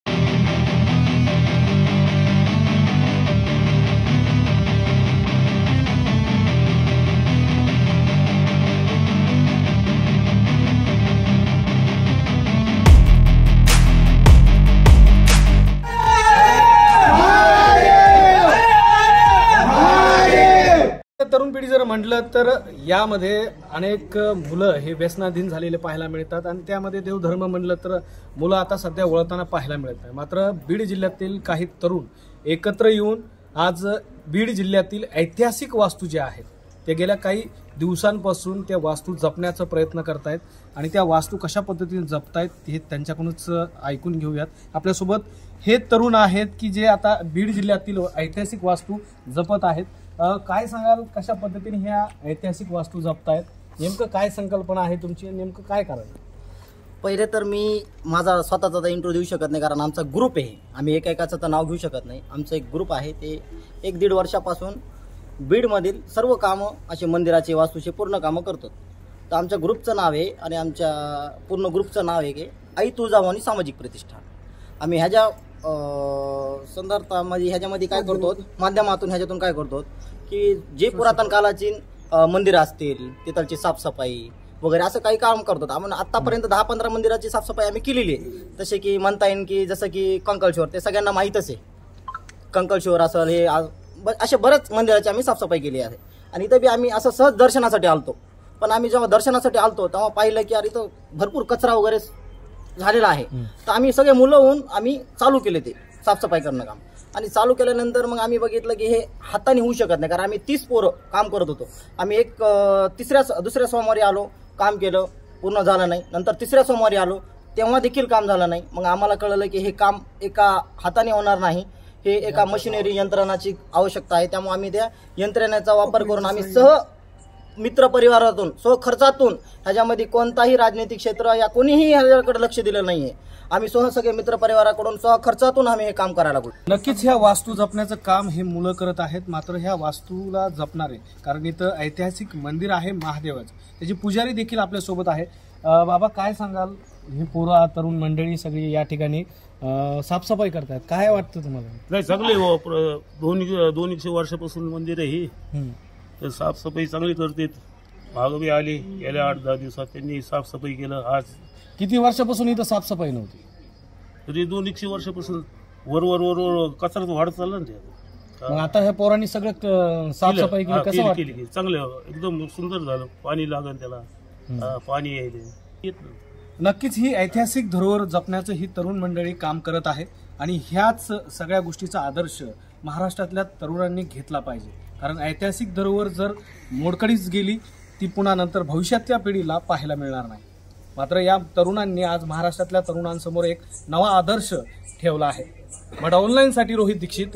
Beed Historic Buildings | तरुणांनी हाती घेतला ऐतिहासिक वास्तू संवर्धनाचा वसा | Maharashtra Times
बीड जिल्ह्यात अनेक ऐतिहासिक वास्तू आहेत यात मंदिर बारवा यासोबतच अनेक ऐतिहासिक पूल देखील पाहायला मिळतात. आता काळानुसार या वास्तूंची पडझड देखील सुरू झाली आहे
मात्र याकडे शासन प्रशासनाचे अक्षम्य दुर्लक्ष होत आहे.
 बीडच्या काही तरुणांनी एकत्र येत या वास्तू जपण्याचा अनोखा निर्णय घेतला आहे.
 गेल्या वर्षभरापासून बीड तालुक्यातील अनेक वास्तू यांनी साफसफाई करत डागडुजी करत आहेत.
आज अनेक तरुण या उपक्रमात  सामील होत असून आपलं श्रमदान करत आहेत.